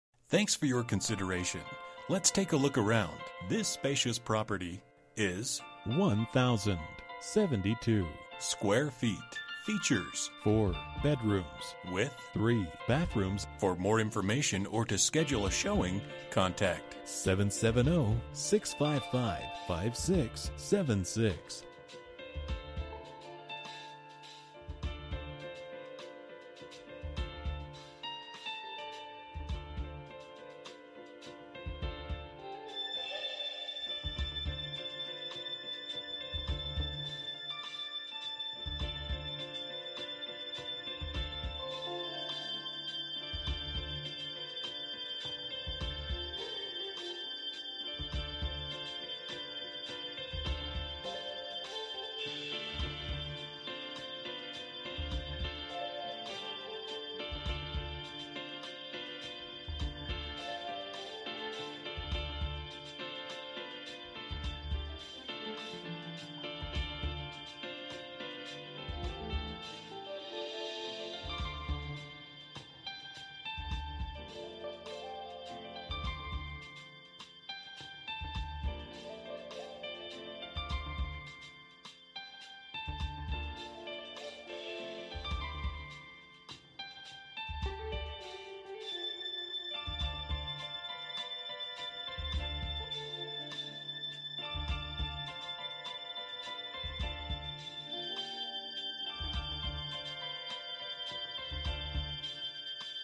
Residential for sale - 11 Woodvine Court, White, GA 30184
Remodeled 4/3 home in Highland Point subdivision conveniently located close to Cartersville, Canton & I75. 3/2 upper level and 1/1 lower level. The perfect layout for teen/in-law suite. Large level lot with plenty of space to park RV, Boat, utility trailer.  No HOA or covenants!

Bedrooms: 4
Bathrooms: 3
Square Feet: 1072
Price: $375,000